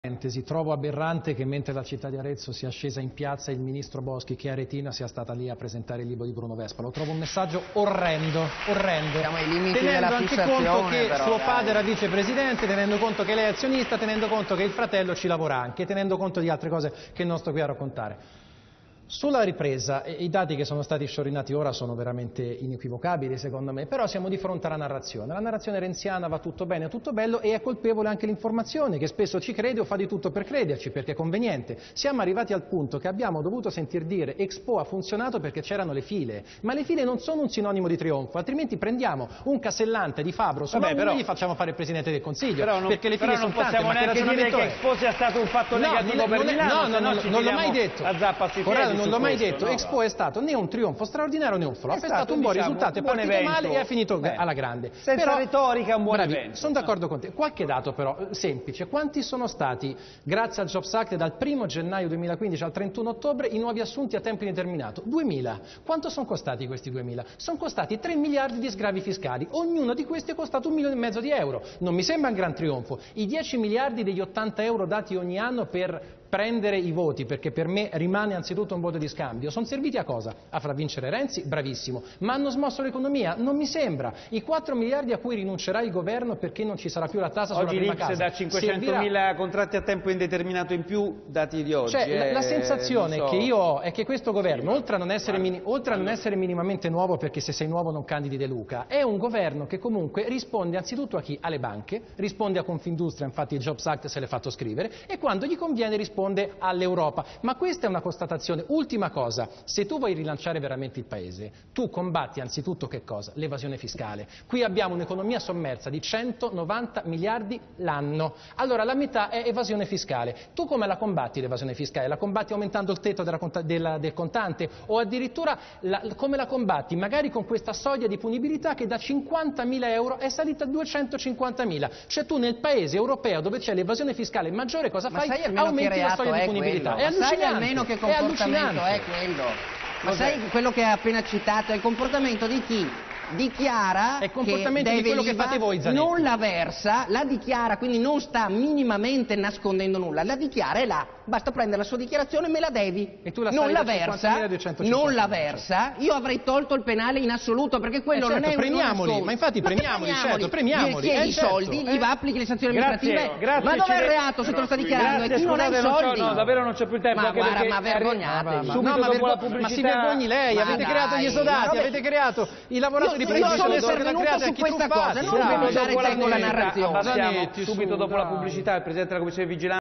Scanzi: 'questo Governo risponde soprattutto alle Banche'. Zanetti 'Si vergogni'
Di parere opposto il giornalista Andrea Scanzi ed Enrico Zanetti, Sottosegretario Ministero dell'Economia, sulla capacità di gestione dell'attuale Governo in materia di economia e finanza pubblica.